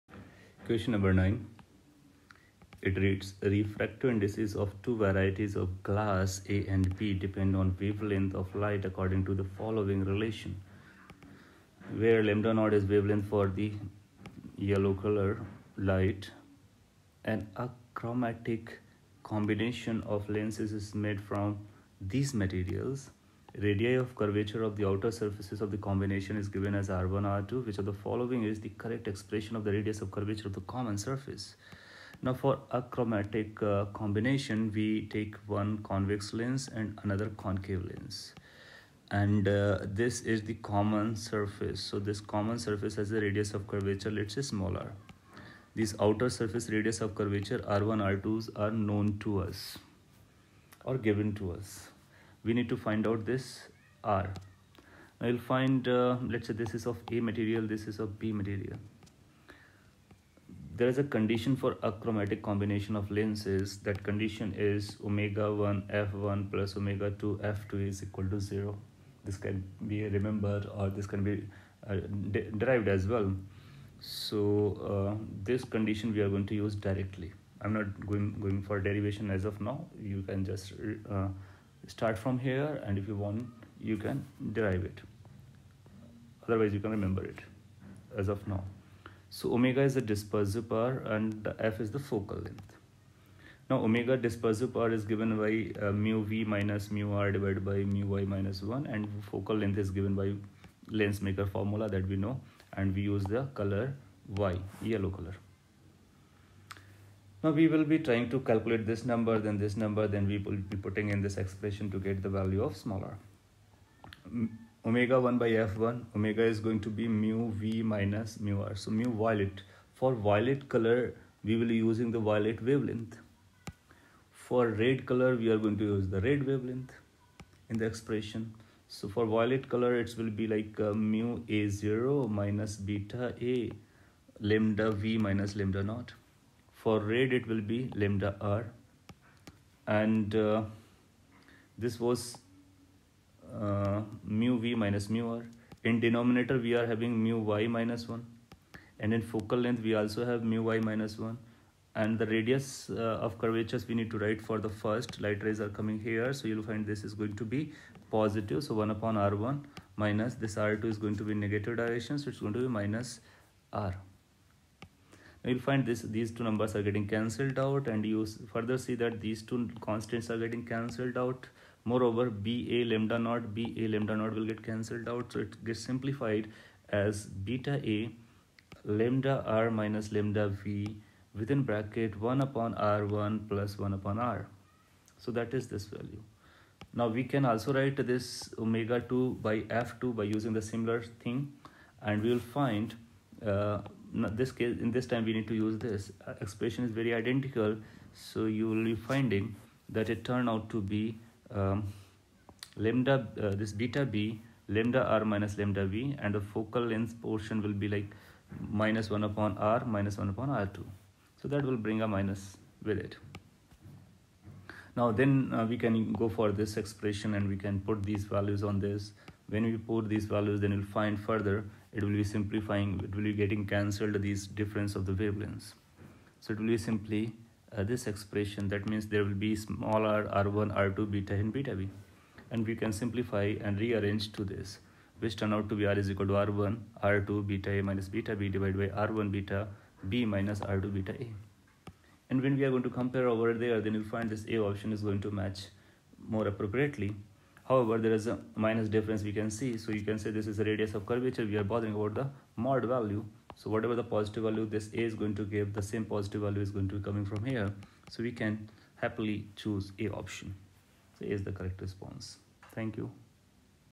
Pathfinder Ray and Wave Optics MCQ 9
Refractive indices of two varieties of glass A and B depend on wavelength lambda of light according to the following equations. Which of the following is a correct expression for radius of curvature of the common surface ?